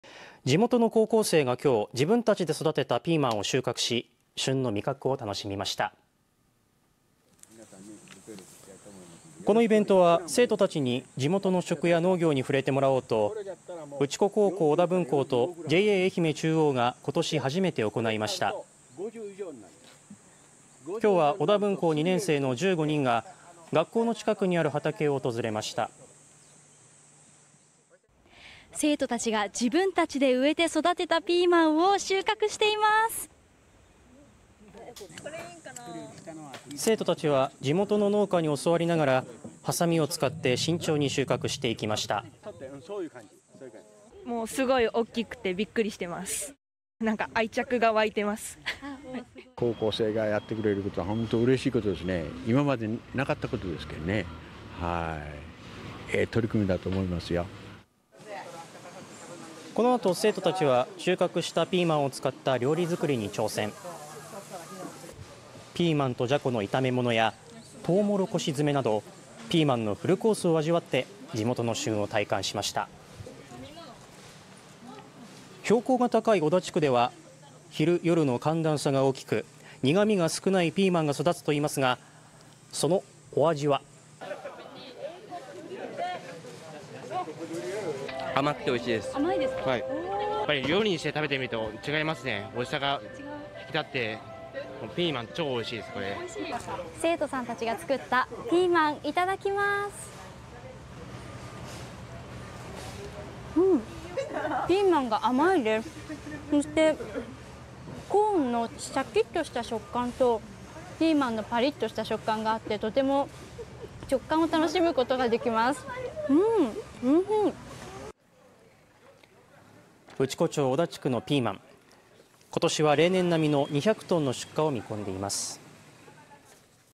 おらが町のピーマンは「おいしさが際立ってる！」高校生が地域の食と農業を学ぶ【愛媛】 (23/07/12 18:00)
内子町小田地区で旬を迎えた特産のピーマン。地元の高校生が１２日、自分たちで育てたピーマンを収穫し、旬の味覚を楽しみました。このイベントは、生徒たちに地元の食や農業に触れてもらおうと内子高校小田分校とＪＡえひめ中央が今年初めて行いました。この日は小田分校２年生の１５人が学校の近くにある畑を訪れました。

曽我部愛麗アナウンサー：
「生徒たちが自分たちで植えて育てたピーマンを収穫しています」

生徒たちは地元の農家に教わりながらハサミを使って慎重に収穫していきました。

内子高校小田分校　西野美桜さん：
「すごい大きくてびっくりしてます。愛着が湧いてます」

ＪＡえひめ中央 ピーマン部会長 泉田孝行さん
「高校生がやってくれることは、本当うれしいことです。今までなかったこと、良い取り組みだと思う」

この後、生徒たちは収穫したピーマンを使った料理作りに挑戦。ピーマンとじゃこの炒め物や、トウモロコシ詰めなど、ピーマンのフルコースを味わって、地元の旬を体感しました。標高が高い小田地区では、昼夜の寒暖差が大きく、苦みが少ないピーマンが育つといいます。果たして、そのお味は？

生徒：
「甘くておいしいです。やっぱり料理にして食べてみると違いますね。おいしさが際立ってピーマン、超おいしいです」

「生徒さんたちが作ったピーマンいただきます。ん～！ピーマンが甘いです。そしてコーンのシャキッとした食感と、ピーマンとパリッとした食感があって、とても食感を楽しむことができます」
内子町小田地区のピーマン、今年は例年並みの２００トンの出荷を見込んでいます。